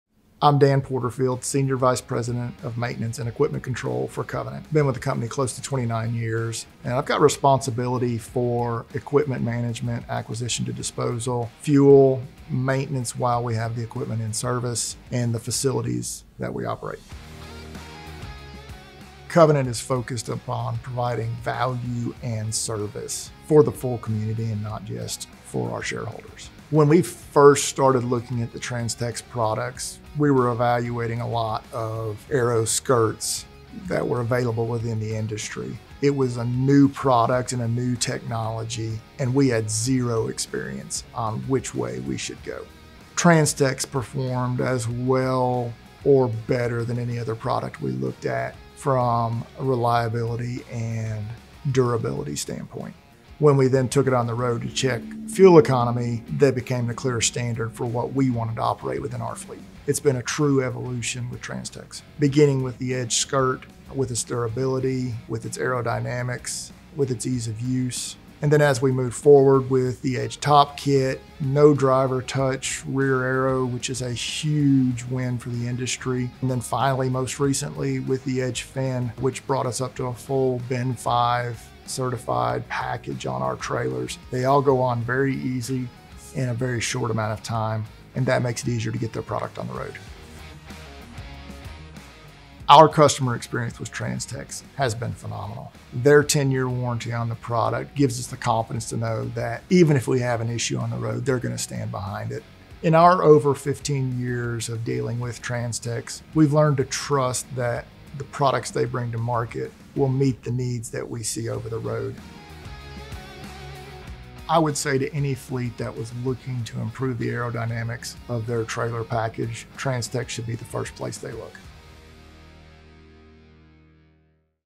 TRANSTEX Testimonial - Dan Porterfield - Covenant Logistics
Dan Porterfield, Senior Vice President of Maintenance & Equipment Control at Covenant Logistics, discusses how TRANSTEX’s innovative line of aerodynamic products enhances fuel economy and reduces carbon emissions. As a TRANSTEX customer for over 15 years, he explains why he trusts our products to meet his fleet's needs and ensure optimal performance on the road.